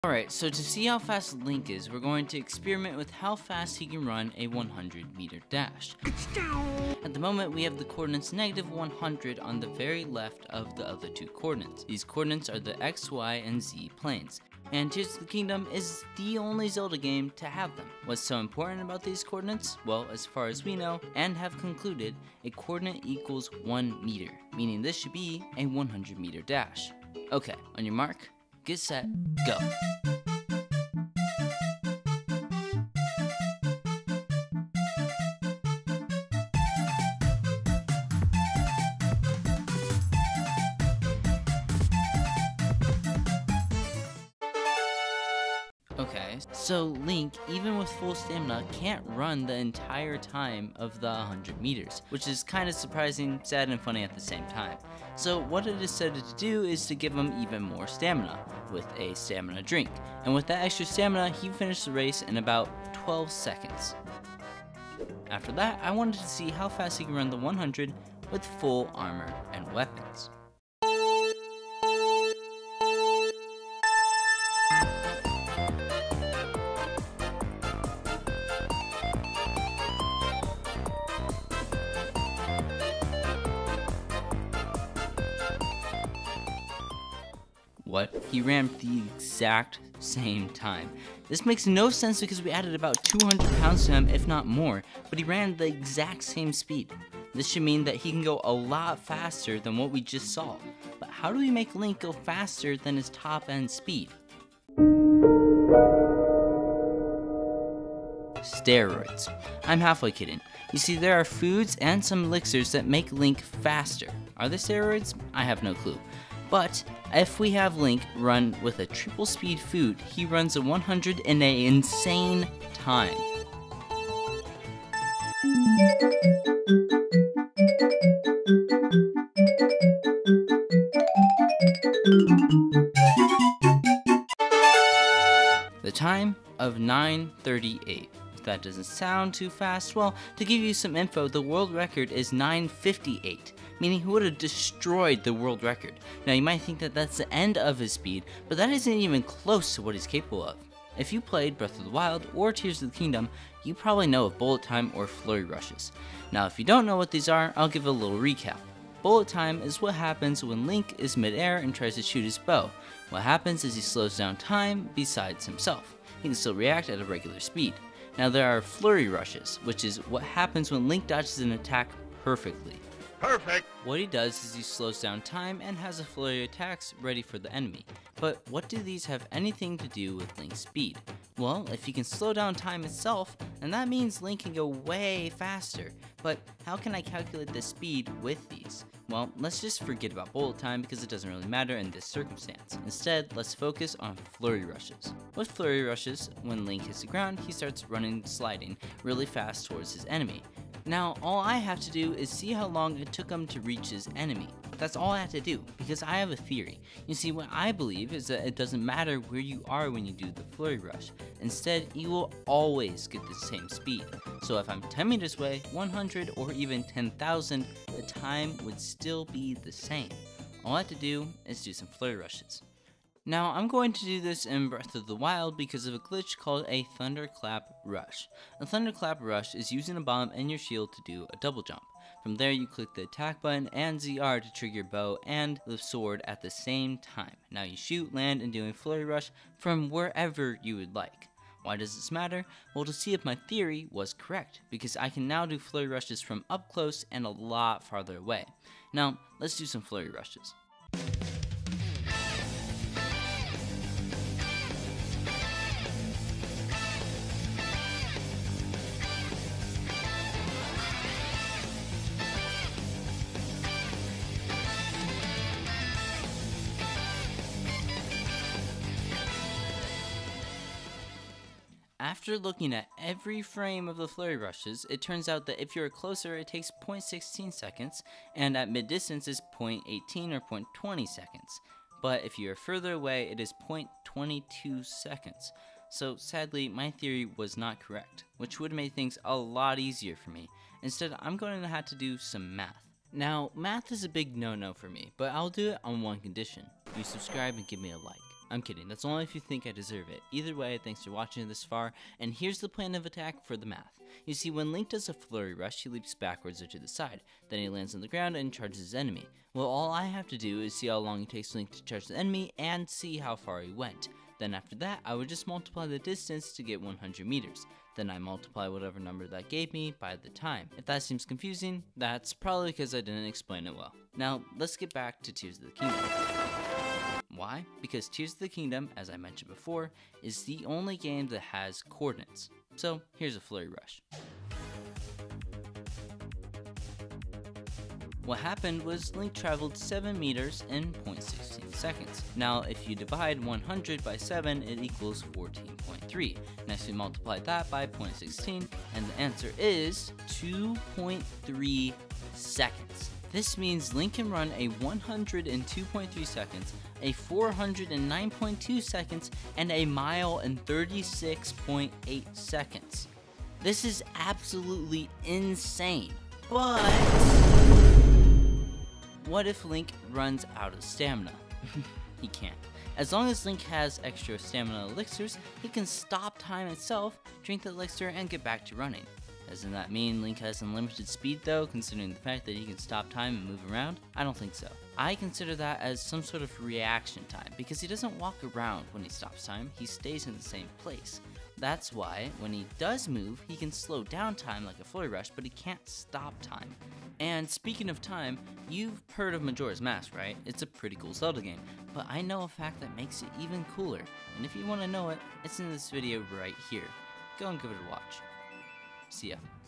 How Fast is Link?
This video talks about how fast Link is, using some math and random crap I tried out.